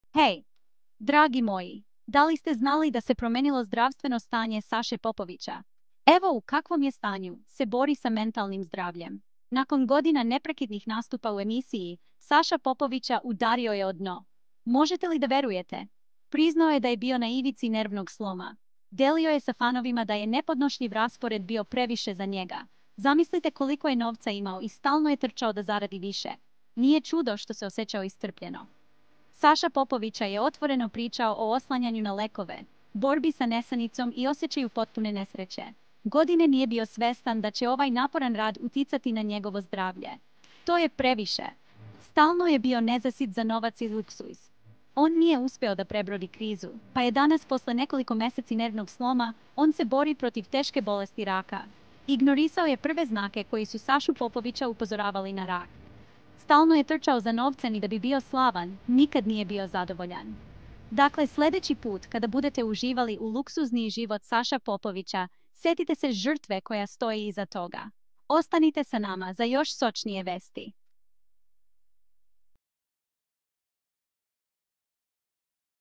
Saša Popović na ivici nervnog sloma, na lekovima i nesrećan!
Saše Popovića na ivici nervnog sloma, na lekovima i nesrećan! -Ne brinite, i dalje volim ono što radim i sve vas, a posebno obožavam da nastupam uživo pred vama Zato ćemo i nastaviti, ali ćemo promeniti način na koji lansiramo muziku!